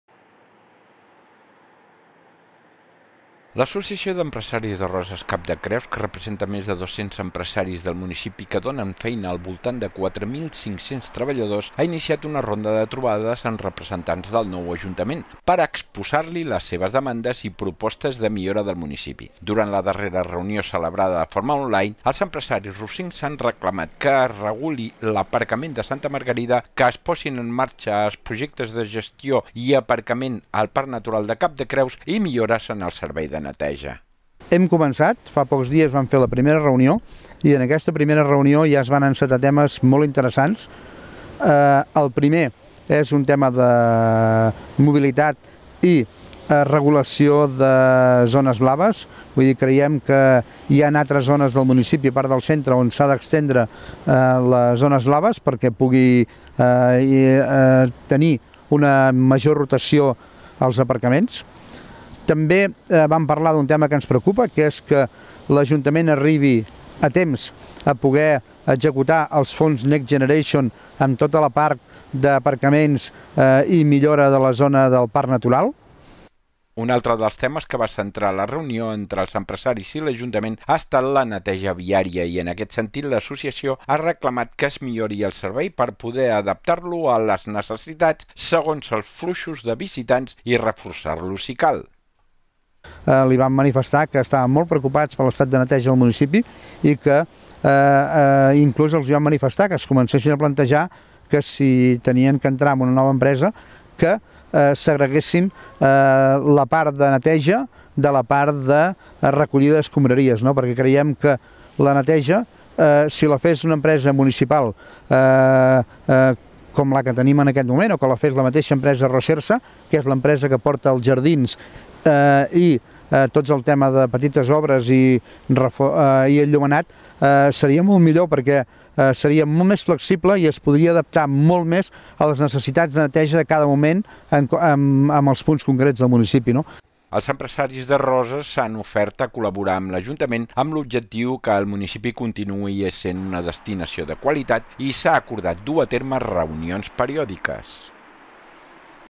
Els empresaris de Roses reclamen a l’Ajuntament millores en la mobilitat i la neteja viària
L’Associació d’Empresaris Roses-Cap de Creus, que representa més de 200 empresaris del municipi que donen feina al voltant de 4.500 treballadors, ha iniciat una ronda de trobades amb representants del nou Ajuntament per exposar-li les seves demandes i propostes de millora,   del municipi. Durant la darrera reunió,  celebrada de forma online, els empresaris rosincs han reclamat que es reguli l’aparcament a Santa Margarita, que es posin en marxa els projectes de gestió i aparcament del Parc Natural  del Cap de Creus, millores en el servei de neteja.

Una altre dels temes que ha centrat la reunió entre els empresaris i l’Ajuntament ha estat la neteja viària, i en aquest sentit l’Associació ha reclamat que es millori el servei per poder adaptar-lo  a les necessitats segons els fluxos de visitants i reforçar-lo, si cal.

Els empresaris de Roses s’han ofert per col·laborar amb l’Ajuntament amb l’objectiu que el municipi continuï essent una destinació de qualitat i s’ha acordat dur a terme reunions periòdiques.